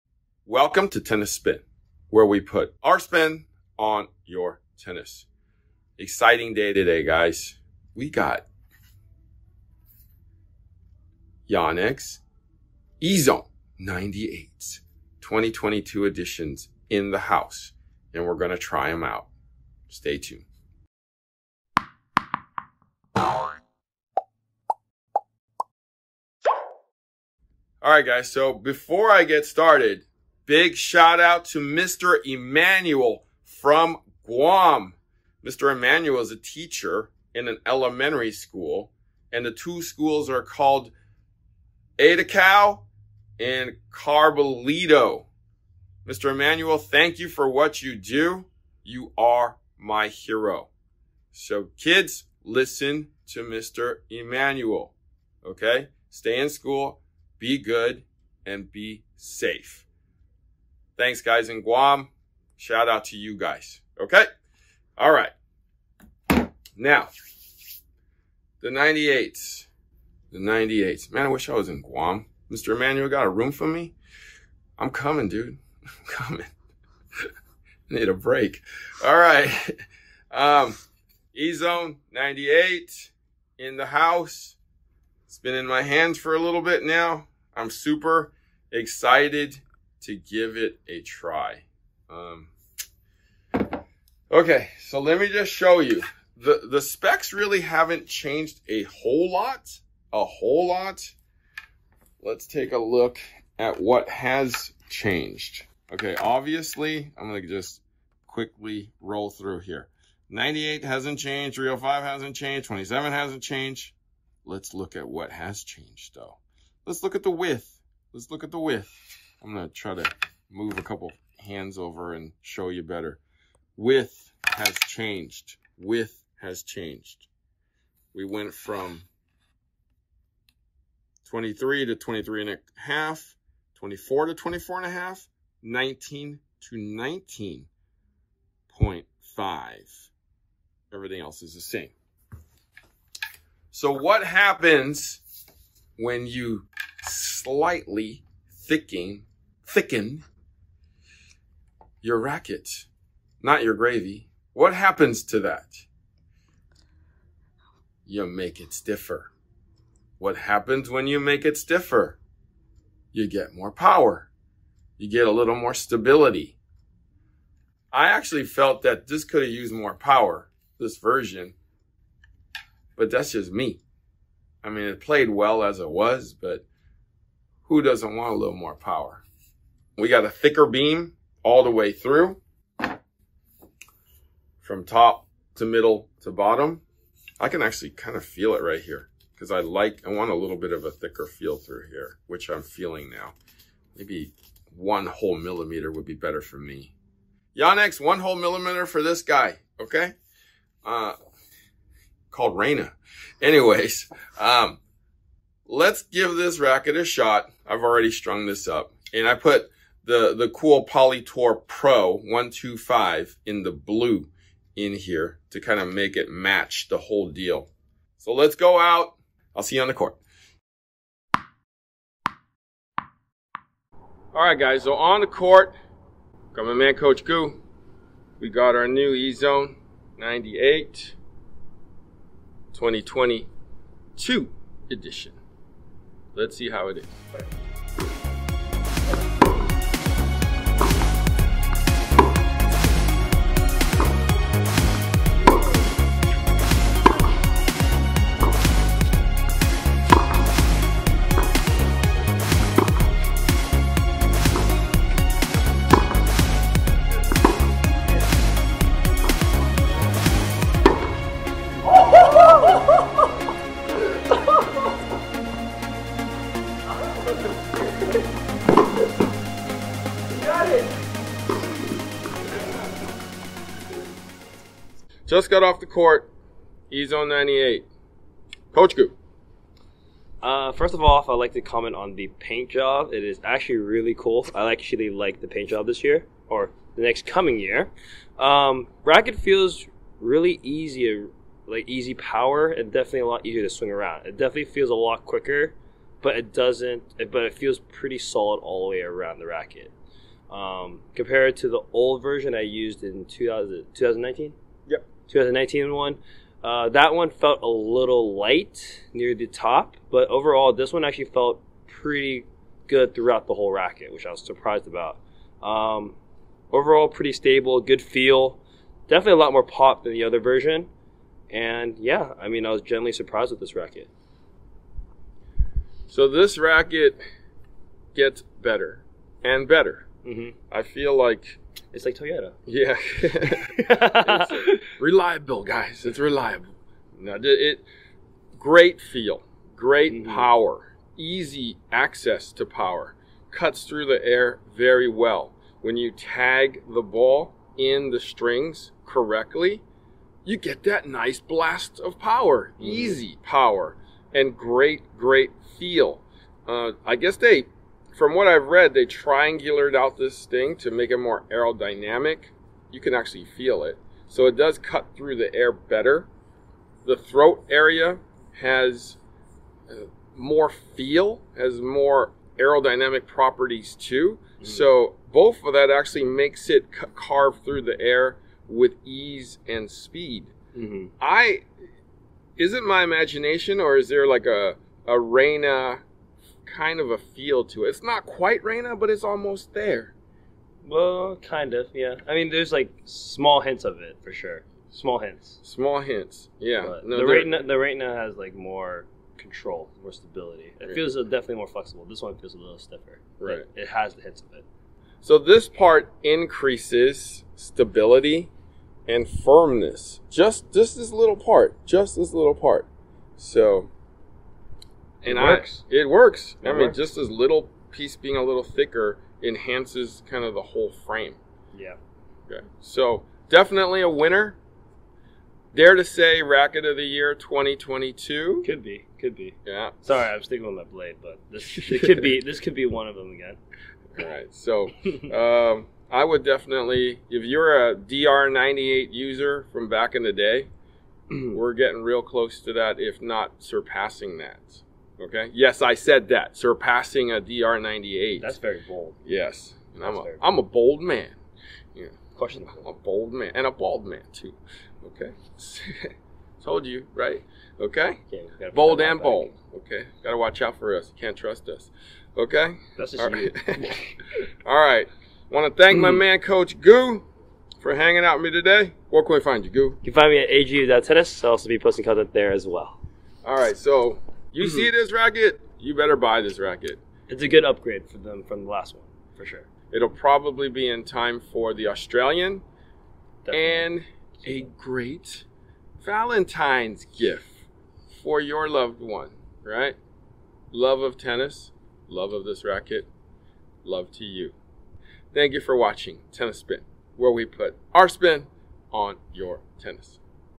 NEW 2022 YONEX EZONE 98 - FIRST PLAY TEST AND REVIEW
Our first playtest of the new 2022 Yonex Zone 98.

Thank you Coach Andrew for trying out this racket with me.

You can follow Coach Andrew Gu on IG @agu.tennis